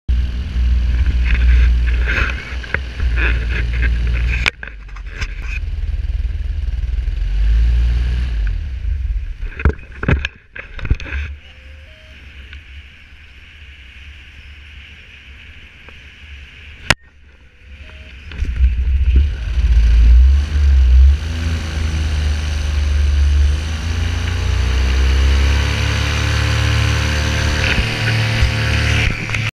Paramoteur , Moster , Redemarage en vol
Arrêt par erreur du moteur, Redemarrage en vol.